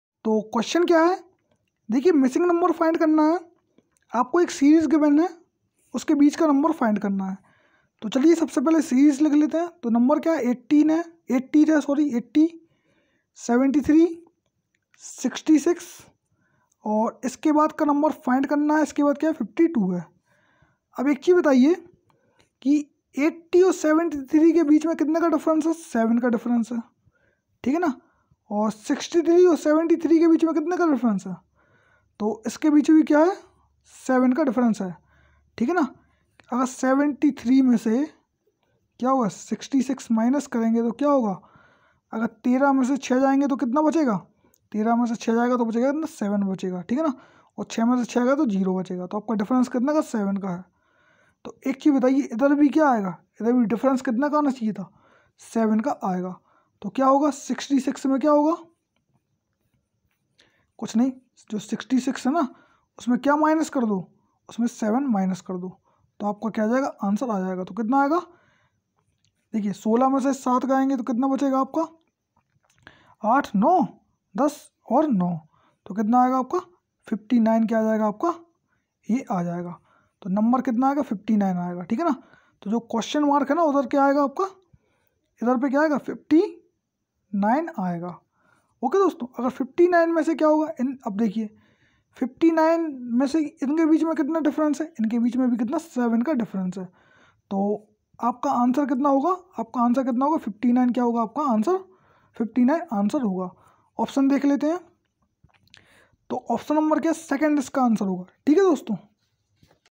Complete the series choosing the missing number 80, 73, 66, ______, 52
Complete the series choosing the missing number
80, 73, 66, ______, 52